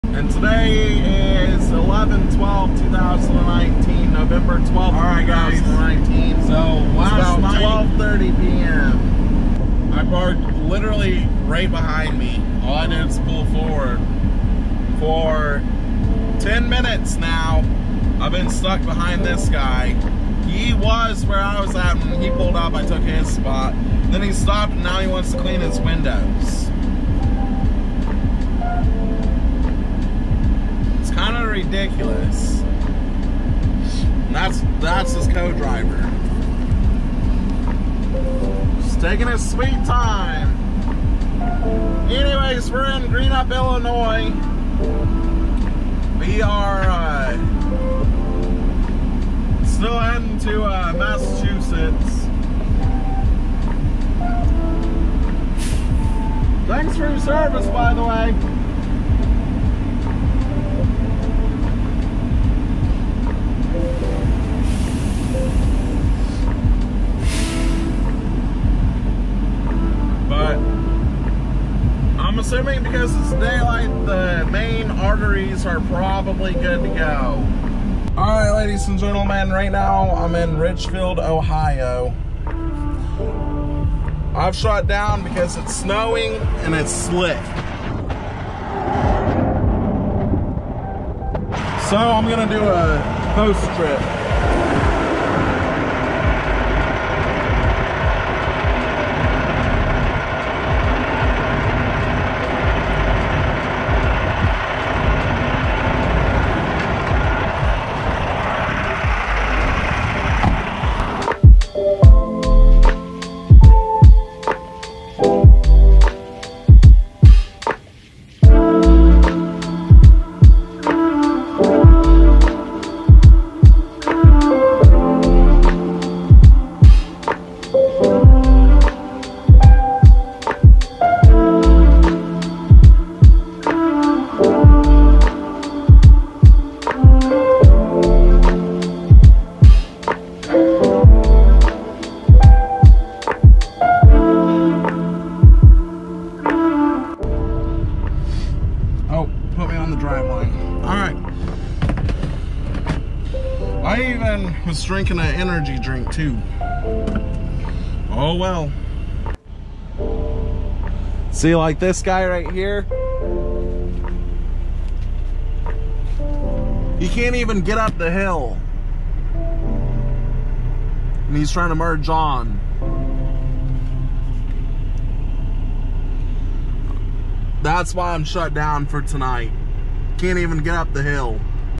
Shut Down Early Because It Was Snowing.
Today I shut down early with over 4 hours left on my clock. I went down a hill and started to slide to the right and the snow was sticking and covering the roads. I'm not going to risk driving in it when I do not feel safe. Loads can be reworked for another time.